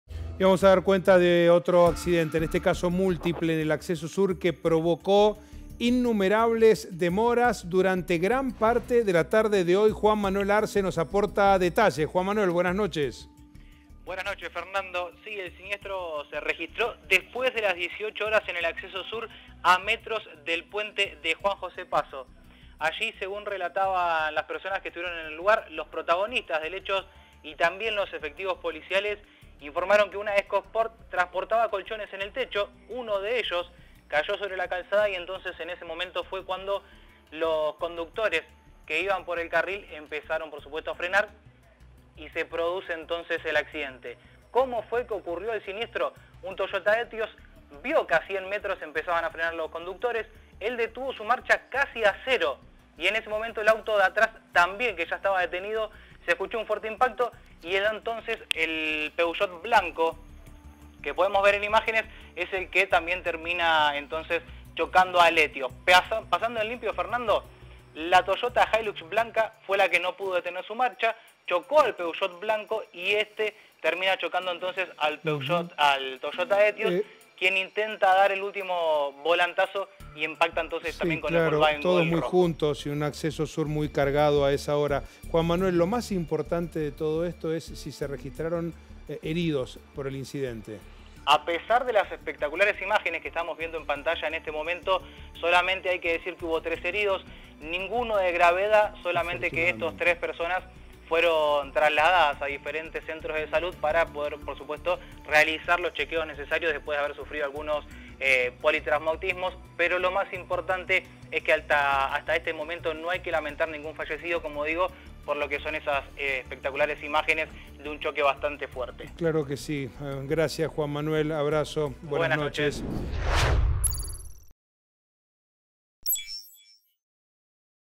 Así fue el choque en cadena que paró el tránsito en el Acceso Sur
Un importante accidente entre varios vehículos se produjo durante la tarde del jueves. El tránsito fue un caos durante varias horas. No hubo víctimas fatales por el incidente.